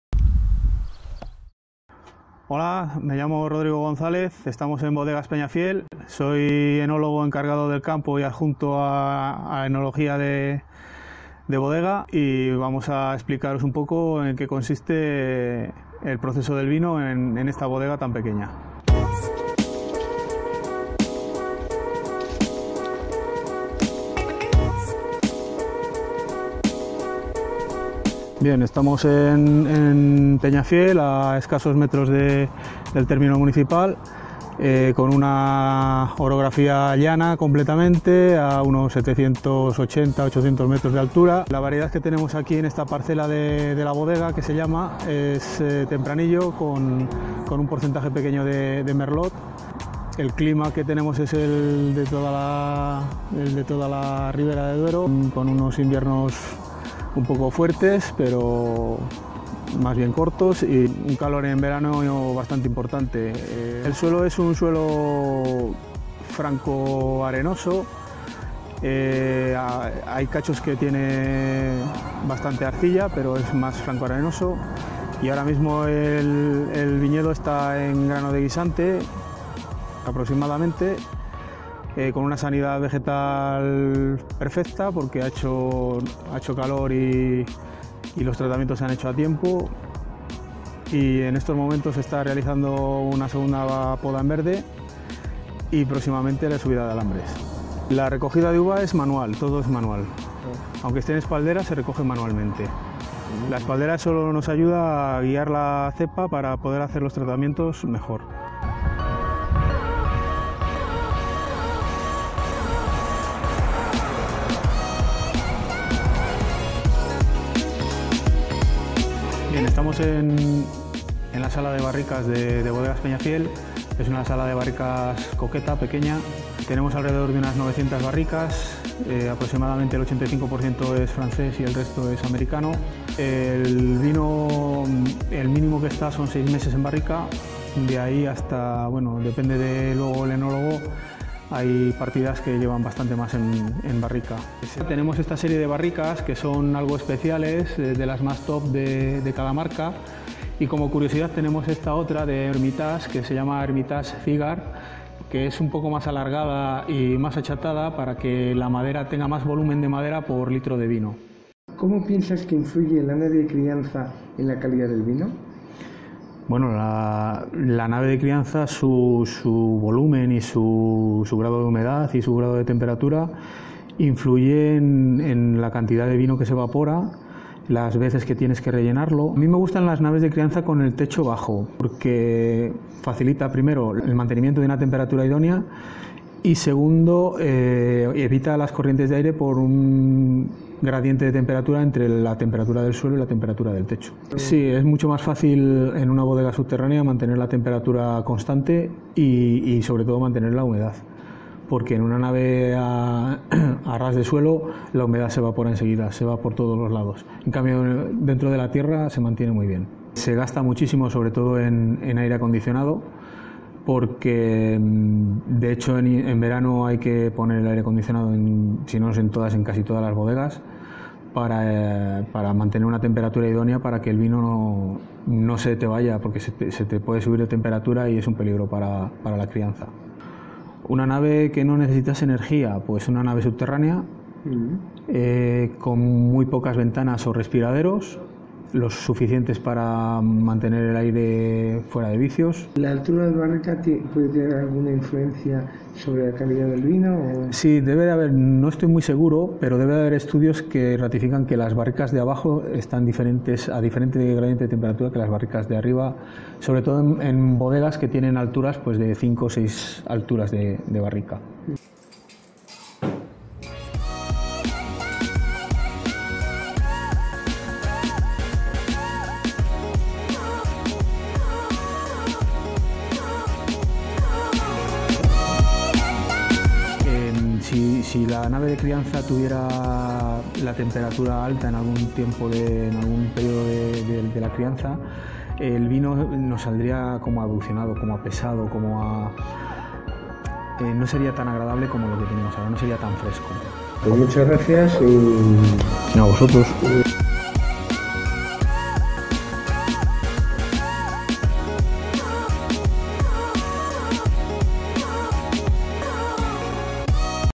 Peñafiel Winery: Wine from Ribera del Duero made in aerial aging cellar with cooling system.
The Peñafiel winery is located at 780 meters above sea level. It has a cold semi-arid climate with some influence of the temperate climate without a dry season and a warm summer. The climate is softened by its location in the Ribera del Duero. The average annual temperature is around 12.1 ° C. In winter temperatures reach -0.1ºC; while in summer thermometers exceed 28.5ºC. Average annual rainfall is 500 mm.